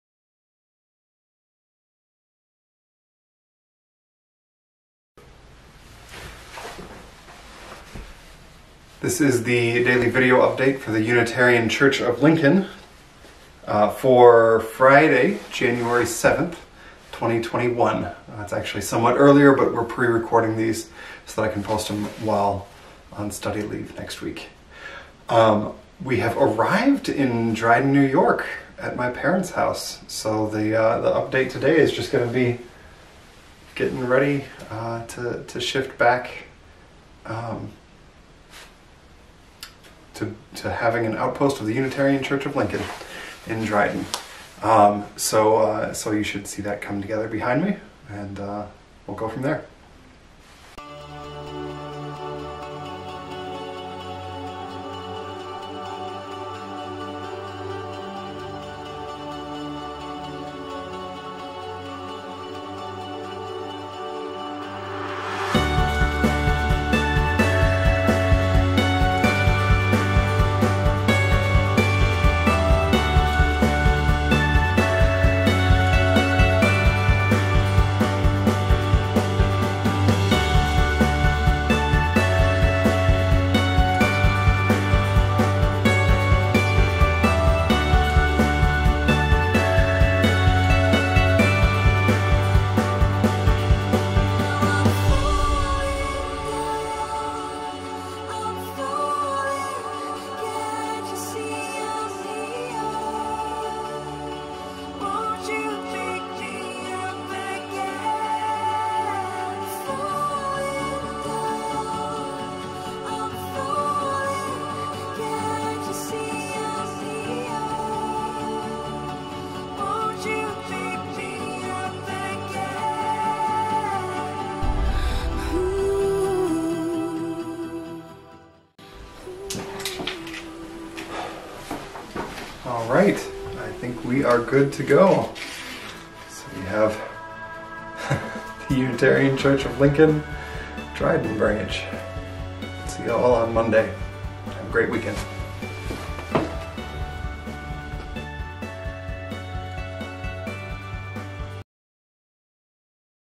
Daily Update January 8, 2021
Sinclair family Roadshow 5/5
Setting up the Dryden branch!